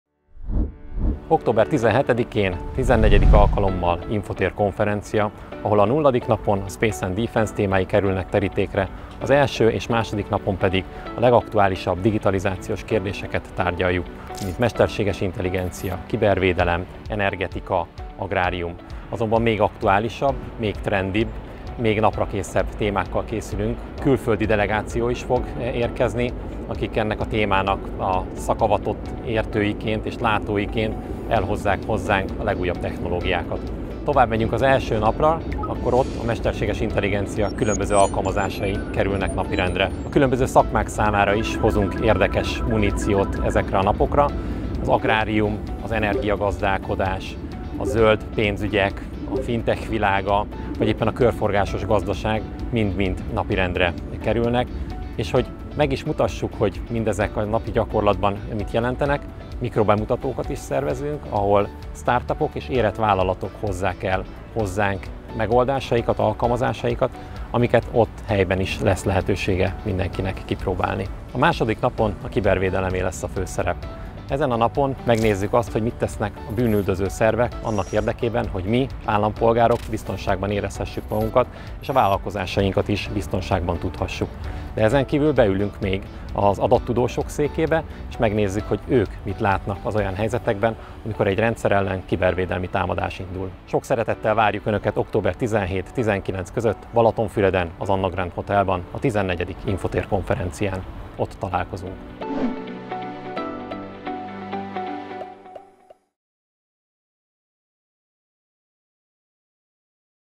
Ilyen lesz a 14. Infotér Konferencia – ugye te sem maradsz otthon?
Október 17-19-én ismét Infotér Konferencia, ahol a 0.napon a Defense and Space, az első és második napon pedig a legaktuálisabb digitális kérdéseket feszegetjük. Kristóf Péter, az Infotér szakértője azt is elárulta, idén milyen újdonságokra számíthatunk.